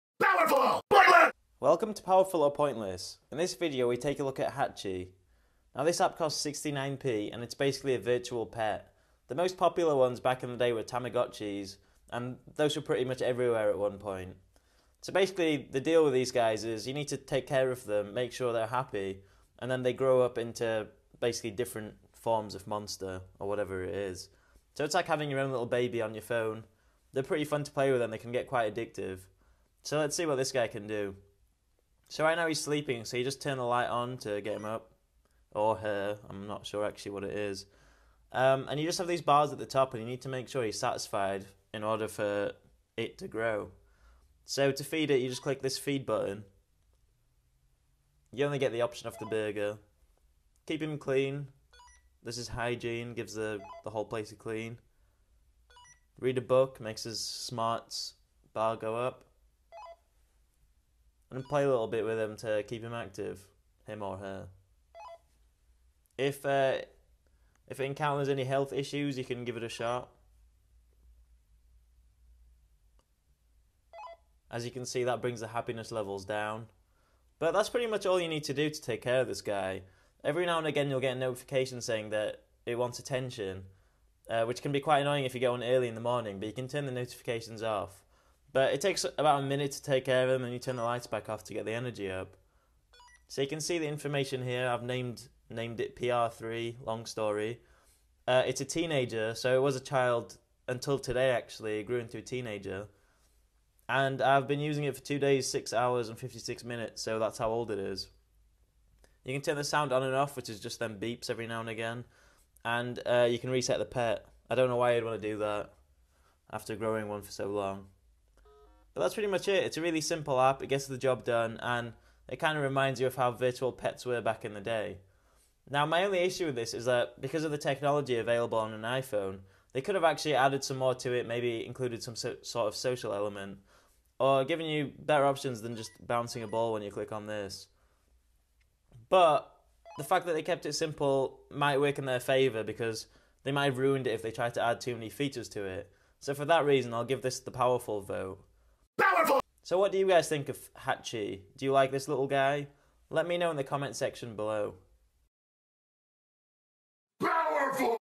Hatchi iPhone App Review
If you want me to review your app, then ask in the comments section!

In this video we take a look at Hatchi. A cute virtual pet by Portable Pixels that costs 69p.

What do you guys think? Powerful or Pointless?